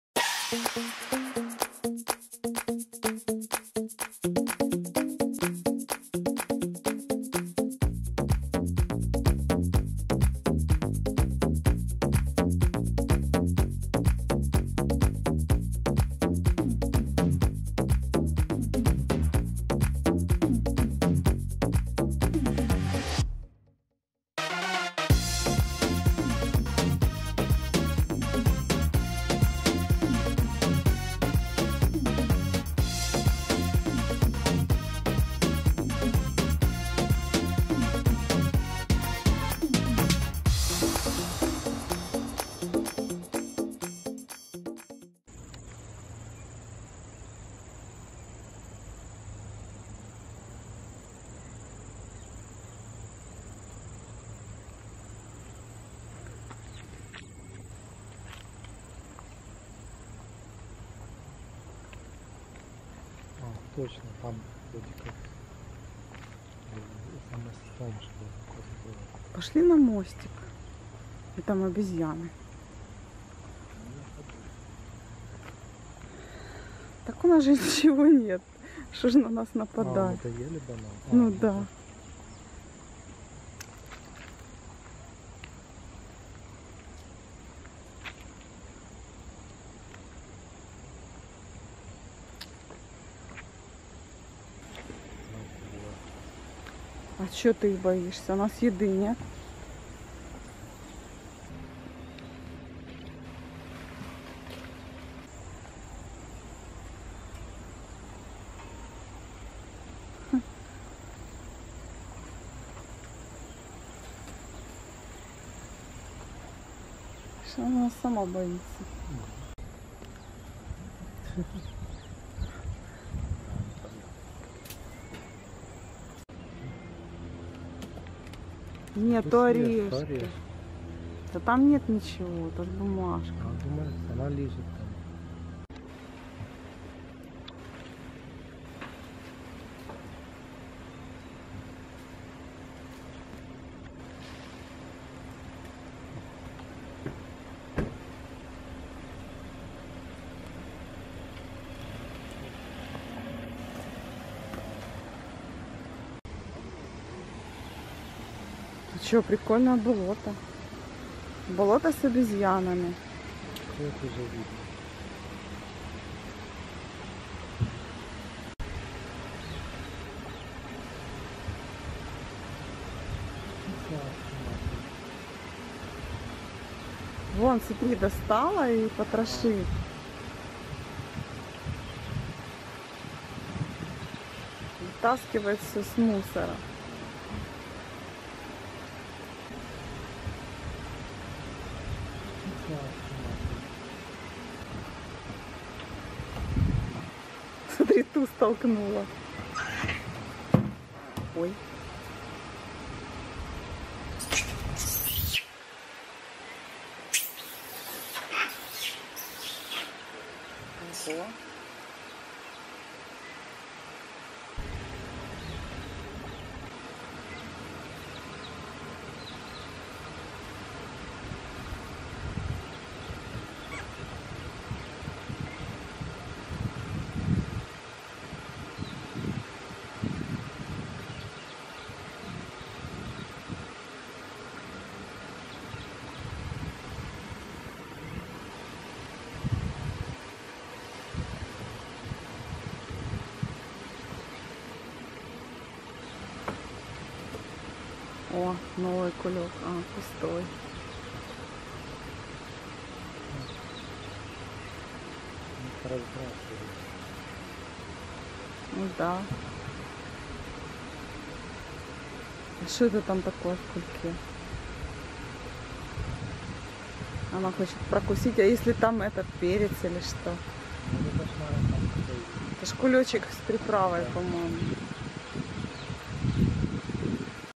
Street Monkeys, Macaque Pranks, Malaysia
Unexpected meeting with monkeys pranksters in the parking lot. We walk from Melaka's ZOO to the Melaka bird park.The monkeys are accustomed to humans. They are not afraid of people. A lot of funny monkeys live in Malaysia.